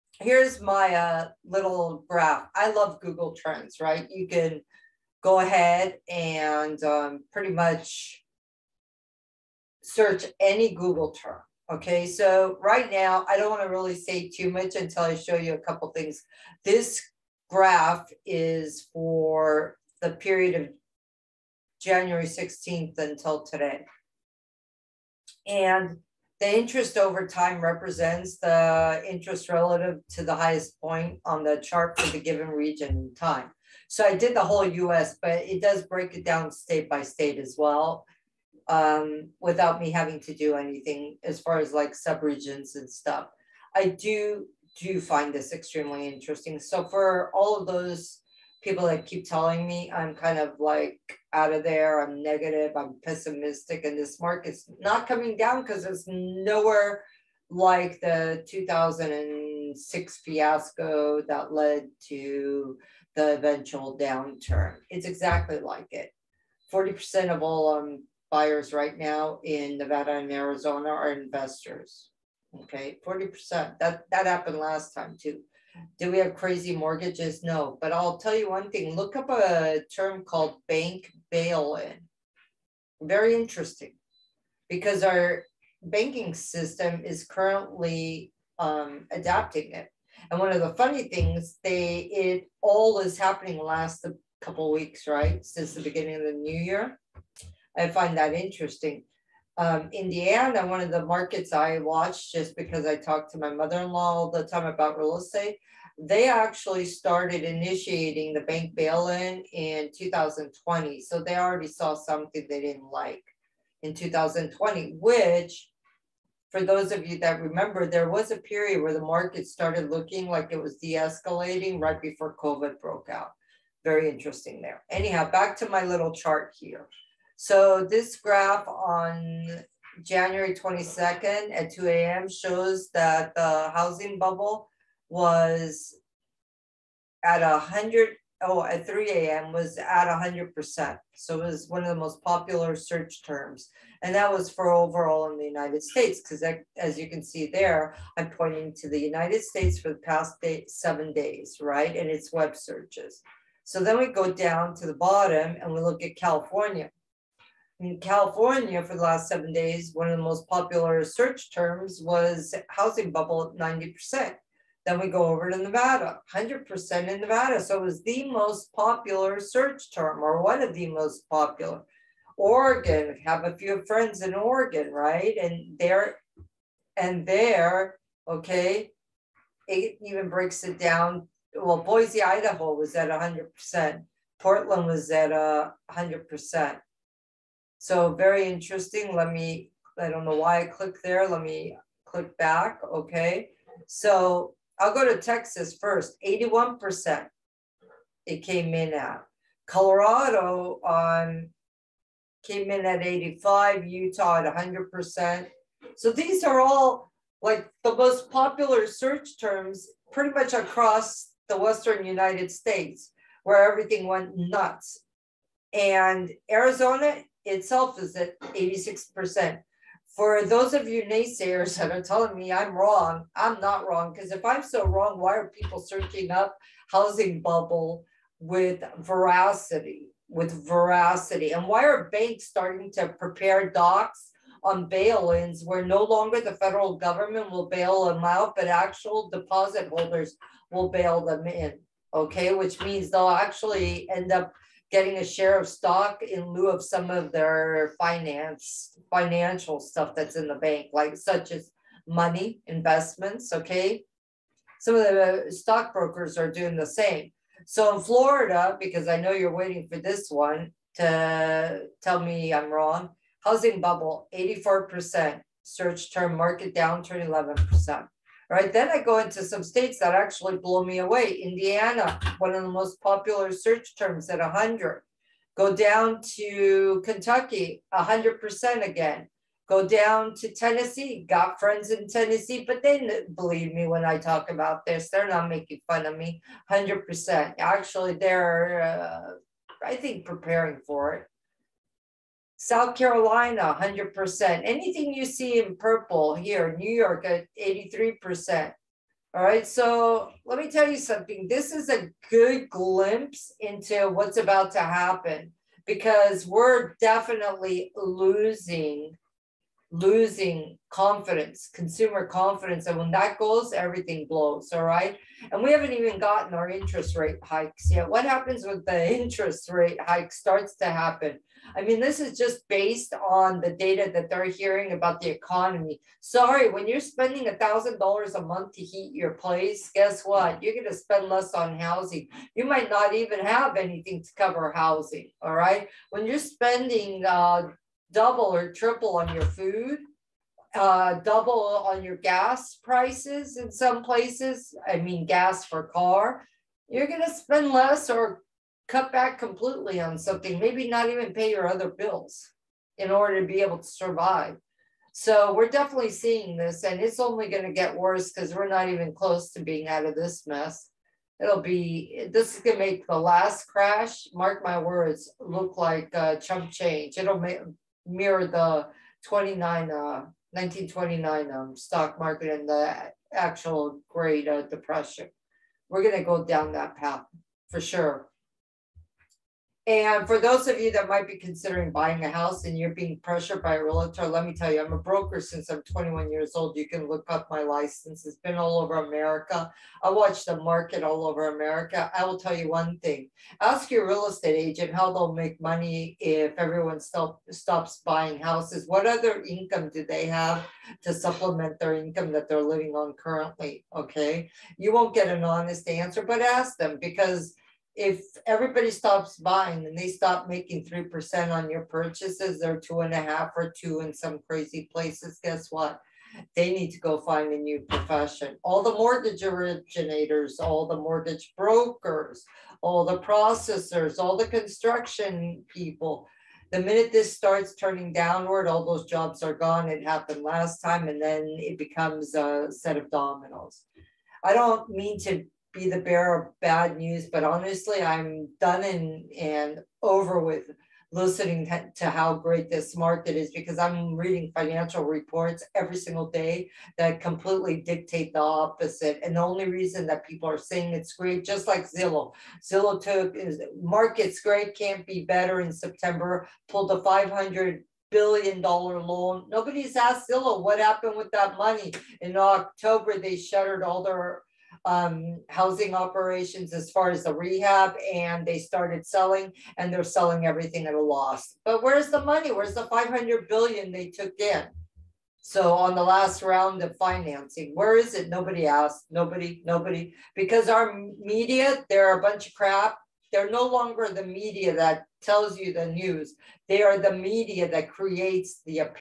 Housing Bubble
Google Searches Don't lie! Check out the rate of google searches in the last 7 days